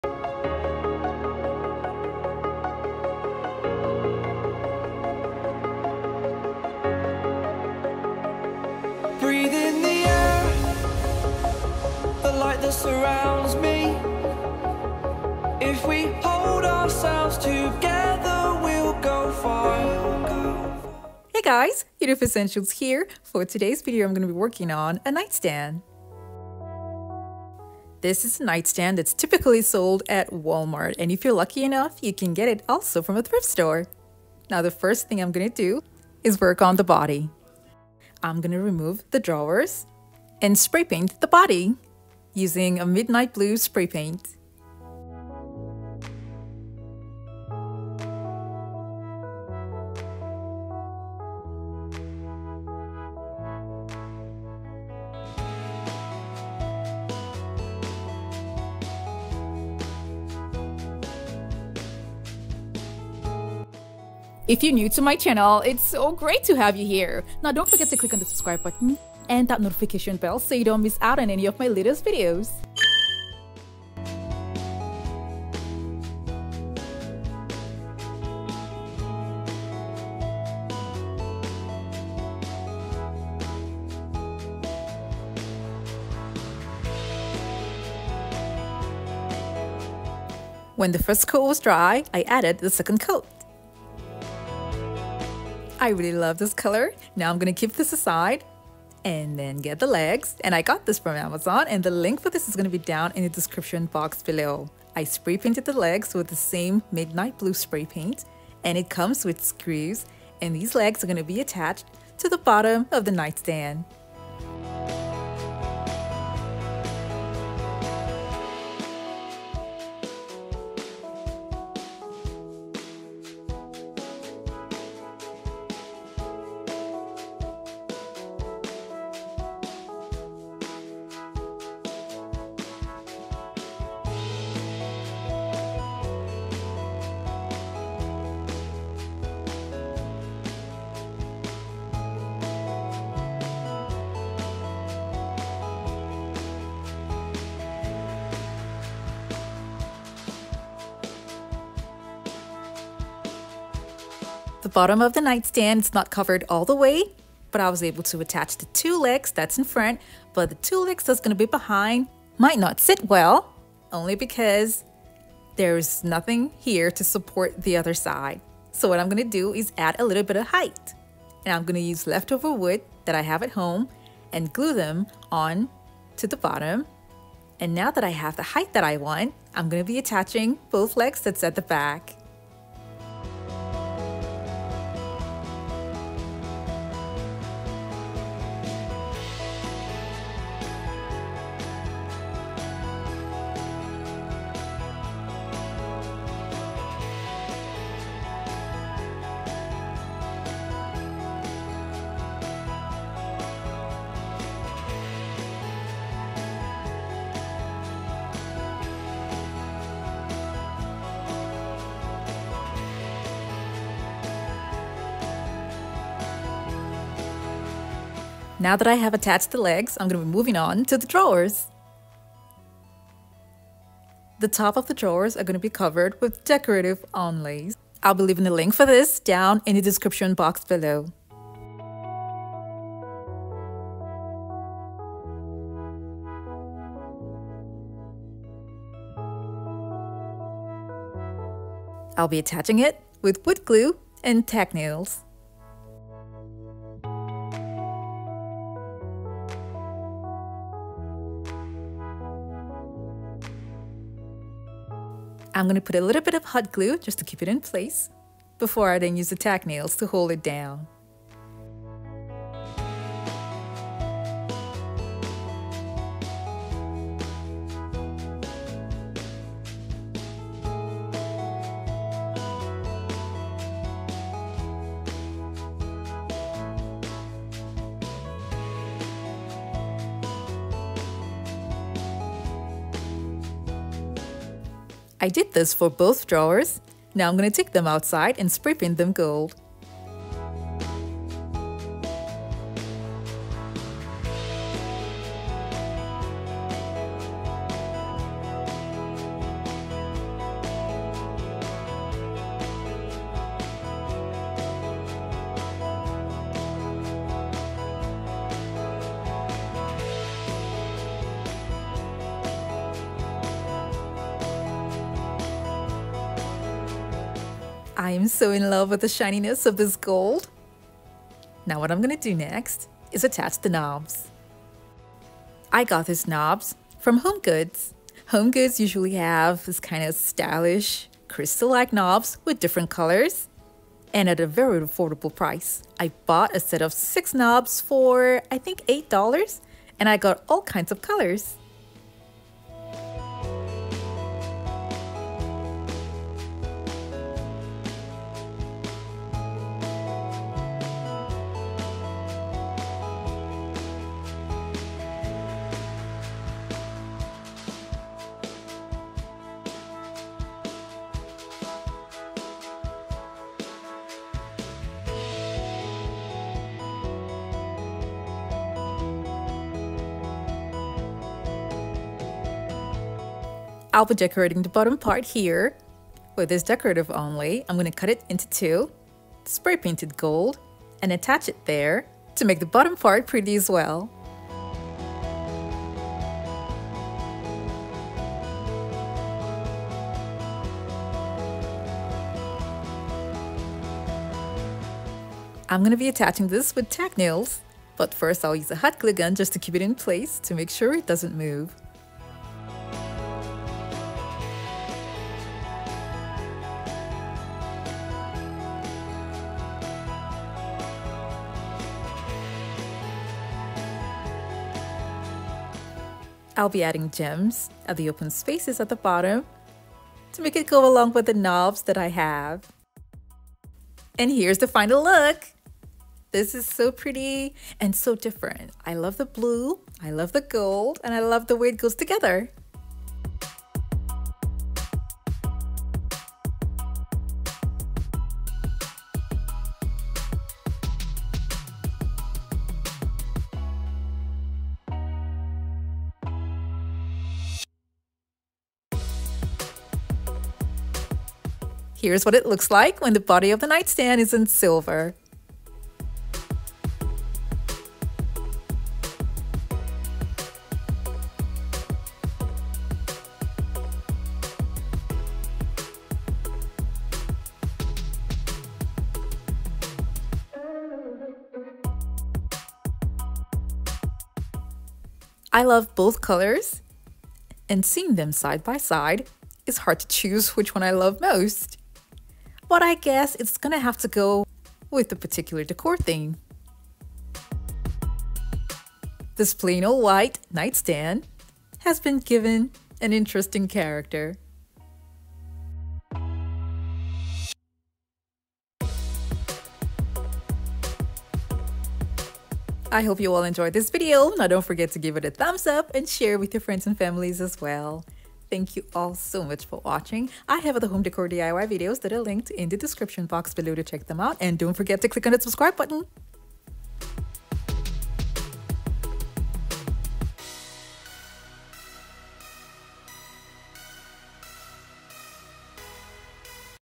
ELEGANT UPDATE TO A PLAIN NIGHT STAND! DIY NIGHT STAND MAKEOVER
CHEAP NIGHT STAND TRANSFORMATION INTO A HIGH END FURNITURE!
This is a Diy Video on How To Transform the Look of a Plain Night Stand into a High End Furniture on a Budget!
How To Pick Casual items and Transforming them to Unique Pieces in a Home is a Continuous and interesting process that I love sharing with others. This is a Home Improvement idea for the Home that is a conversation starter and It is also Budget Friendly as well!

HERE ARE THE ITEMS USED:
White Night Stand
Midnight Blue Spray paint
White Decorative Onlays
Smaller Corner Onlays
Nightstand Legs

yoduvh_diy